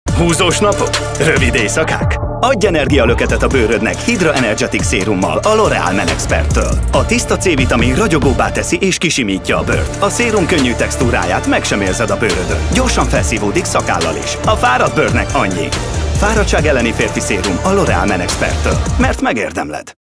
L'Oréal Men Expert: Hydra Energetic szérum
Napi energialöket a bőrödnek: a Hydra Energetic szérum 10% tiszta C-vitaminnal a ragyogóbb, kisimult és frissebb hatású arcbőrért. Ráadásul nem ragad, nem zsírosít, és szakáll alatt is könnyen felszívódik. Csak vigyél fel pár cseppet, masszírozd be, és mehetsz is!

Kövess minket Facebook-on!